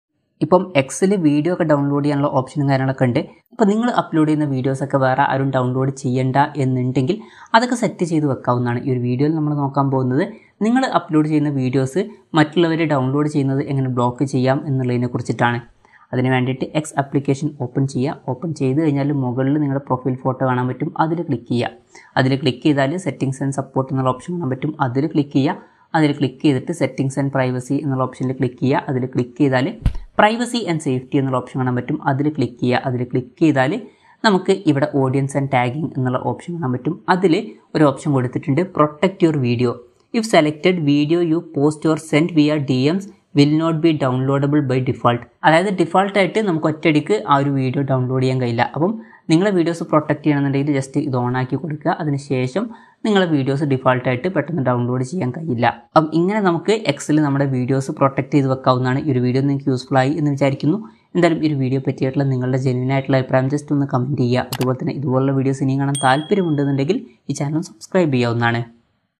How To Protect Your Videos In X ( Twitter ) | Malayalam
in this video we will be telling about how to protect your videos in x ( twitter ) in malayalam. how to restrict others from downloading your video in x or twitter. lets check about how to protect your videos in x or twitter in malayalam

my gadgets